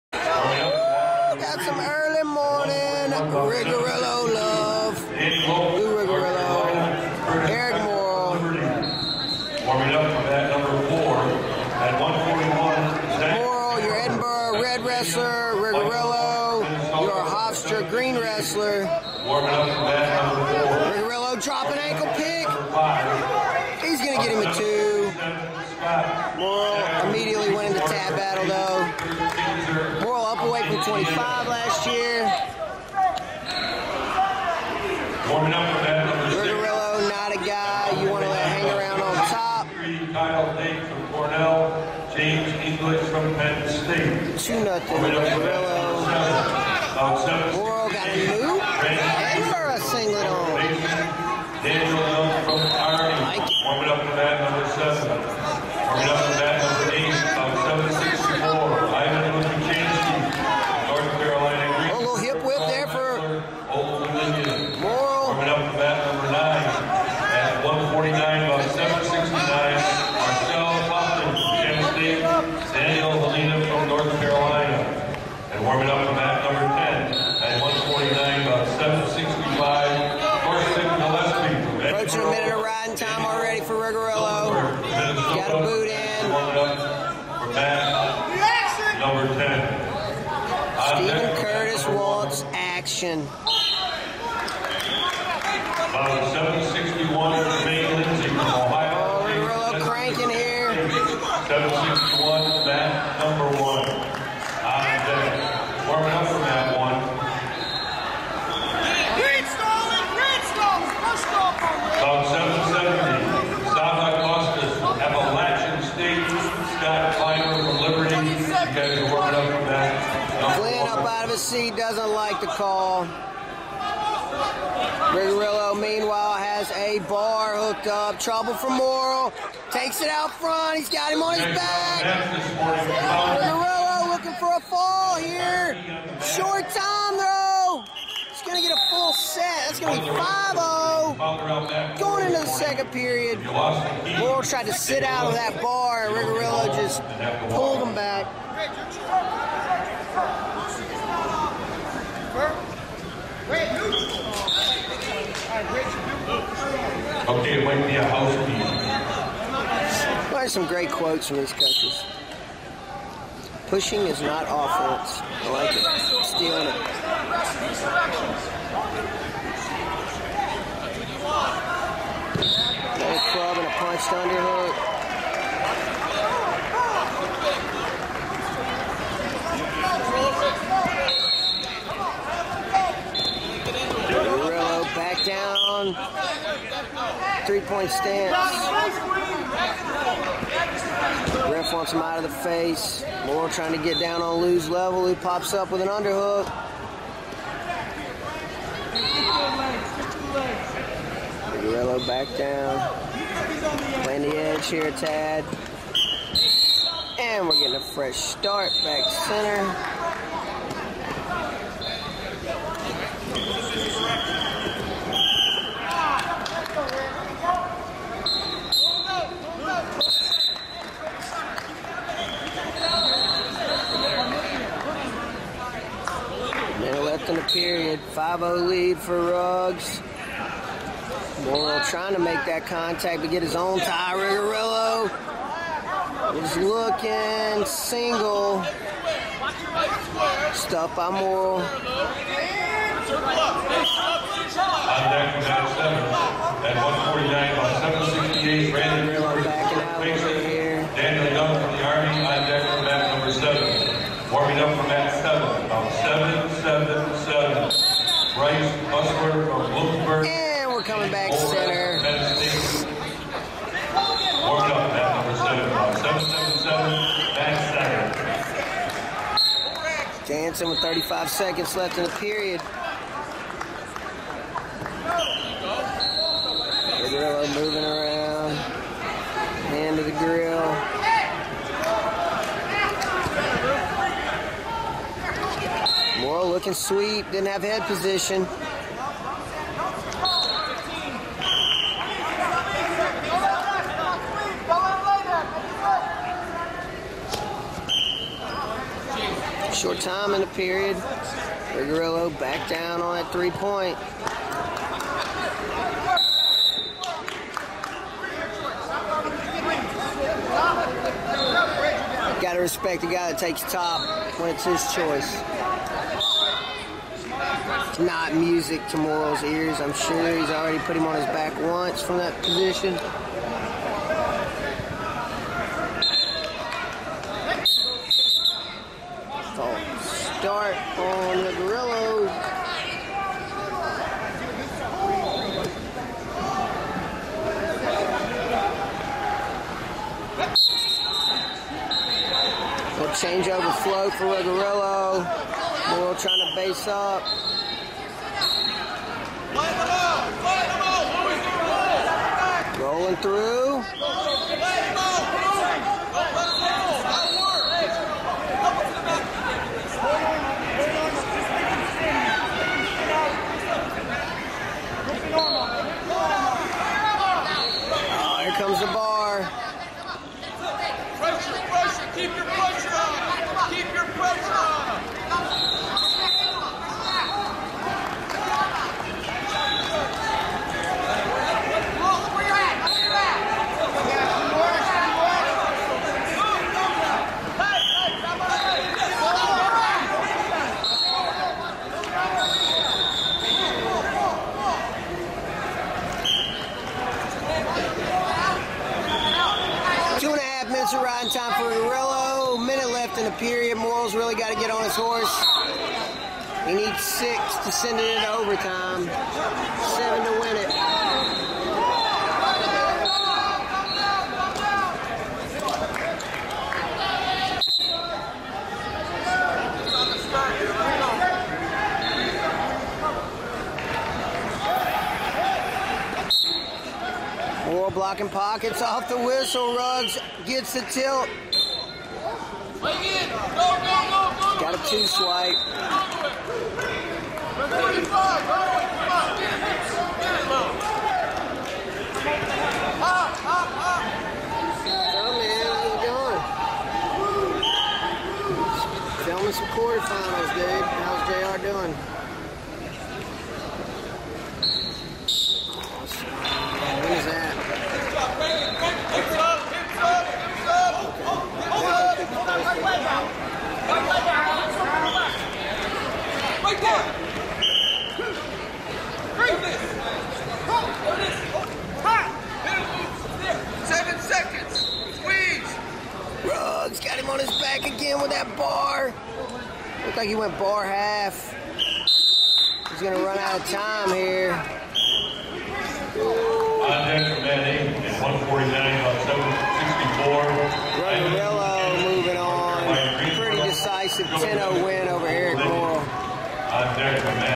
133 lbs quarter finals Lou Ruggirello vs Eric Morill Edinboro
The Southern Scuffle was and has remained one of the premier regular season events in college wrestling. After UNCG dropped their wrestling program UTC took over this prestigious event and it continues to bring in some of the top talent and match ups in the history of the sport.